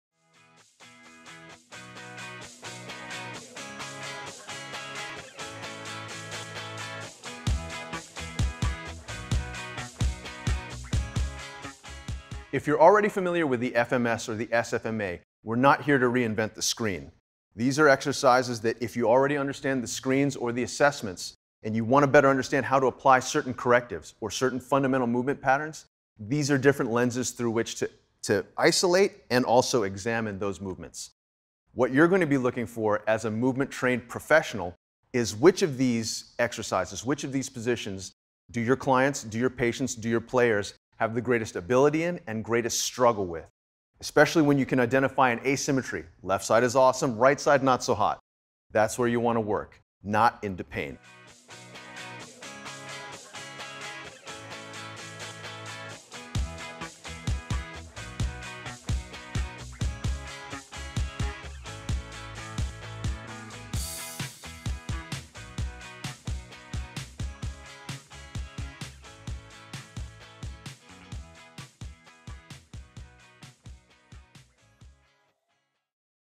Dr. Mark Cheng: How Does Prehab-Rehab 101 Fit into the FMS & SFMA?
Many coaches, trainers, and fitness professionals have heard of the Functional Movement Screen or FMS as a tool for risk management, which allows the user to screen for asymmetries in movement patterns that can serve as indicators of movement dysfunction and potential injury risk. The clinical version of the FMS is the Selective Functional Movement Assessment (SFMA), also based on 7 top-tier tests to help identify the "lynchpin dysfunction" behind the pain.

These 2 tools have become game-changers in the fields of strength & conditioning, athletic training, and pain rehabilitation. As they've allowed professionals to examine movement as a series of coordinated patterns instead of merely isolated body parts, the FMS & SFMA are dramatically changing the way we exercise and train.

One of the key concepts in Functional Movement Systems is the emphasis on addressing the movement pattern which has the greatest asymmetry and allowing the client or patient to work on developing competency in that pattern as long as there is no pain with that movement.

Prehab-Rehab 101 is focused on explaining, demonstrating, and then demystifying 5 of the most fundamental positions that are part of the human developmental process. The video gives the user very clear bullet points to meet for the precise set-up of each position. Each of the 5 positions has direct tie-over to different facets of the FMS and SFMA, explaining some of the corrective exercises that can improve the stability, mobility, and coordination of certain joints as they strive to play in concert.

As Dr. Cheng covers each exercise, he also offers several "points of entry", allowing for exploration of the movement pattern with minimal threat or stress while maximizing the neuromuscular learning opportunities & benefits. So when the viewer finds a particular movement or exercise where there is a notable difference in left-right ability or ease, that's one of the possible points of entry into addressing asymmetry and allowing the brain to "re-map" the precision behind the movement.